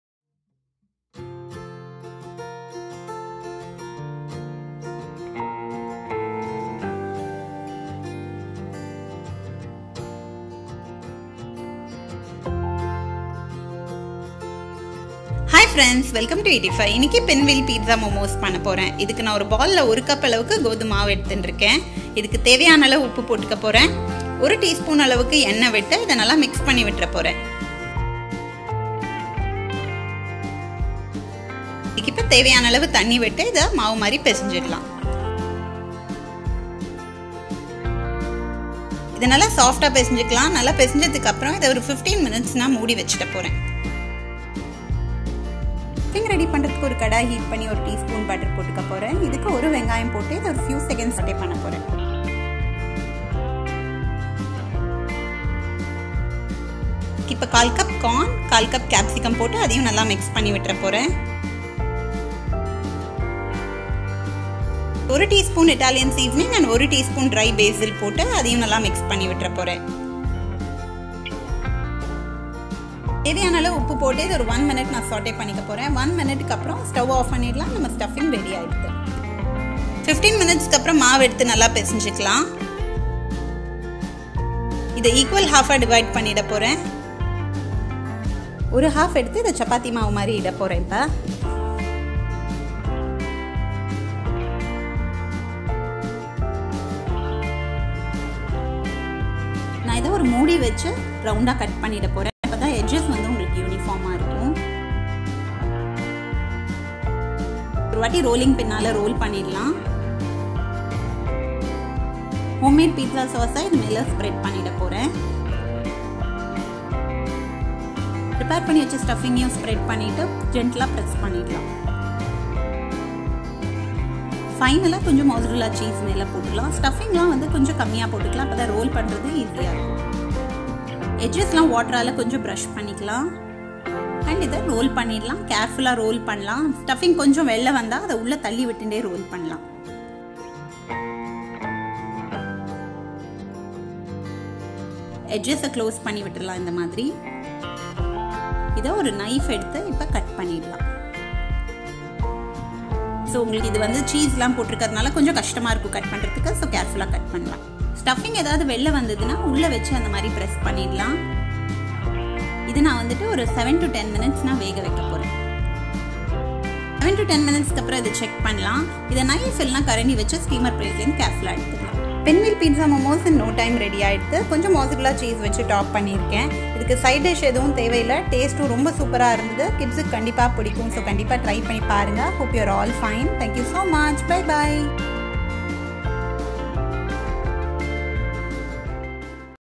Pinwheel Pizza Momos Recipe - பின்வீல் பிட்சா மோமோஸ் - Veg Momos - with English Subtitles
Hi Friends,
Hope you are all doing fine. Any momo and pizza lovers out there. How tasty it will be when both come together😋Hands down kids will not say a no to this. Why wait please do check out the full recipe below. ❤️Keep Supporting. Stay Healthy😊Stay Happy😊❤️